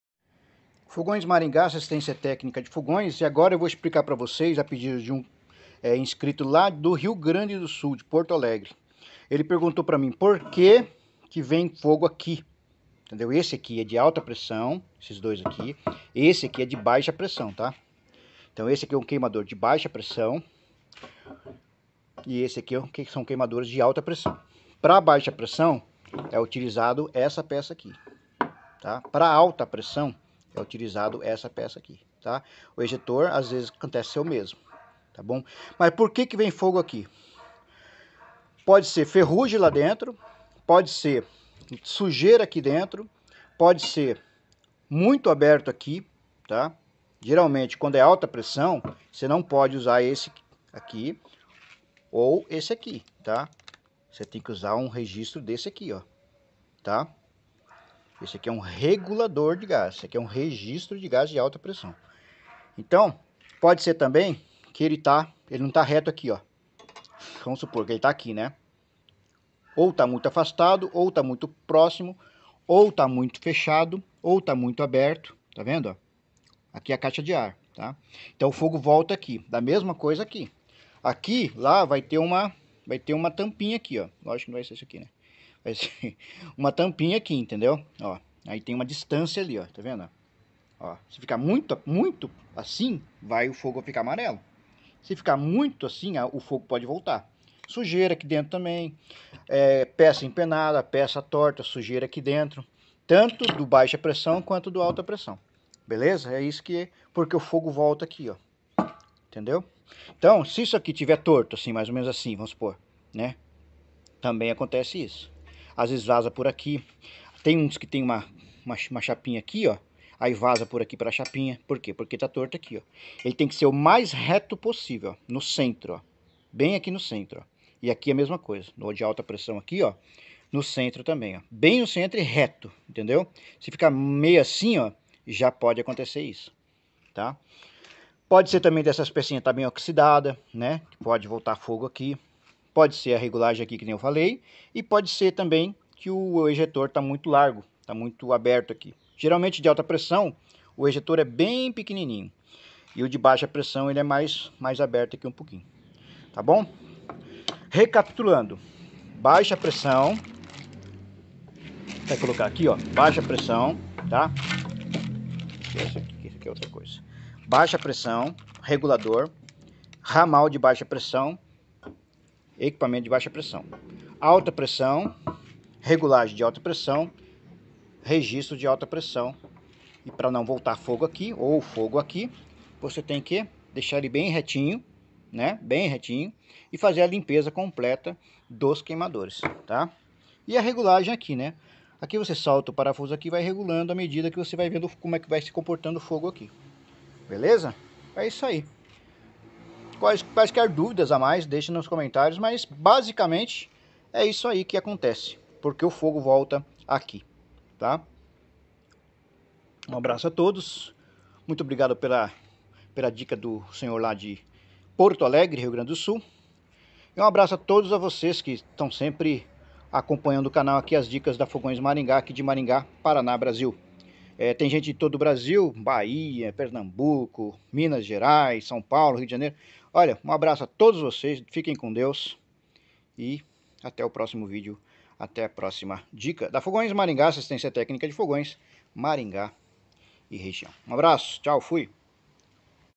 Por que o fogo volta no cano do queimador? Eis a explicação. Fogões Maringá
Um inscrito me questionou sobre o por que que isto acontece e eu resolvi fazer este vídeo para ajudar e tirar a dúvida de vcs. O por que volta fogo no cano do queimador.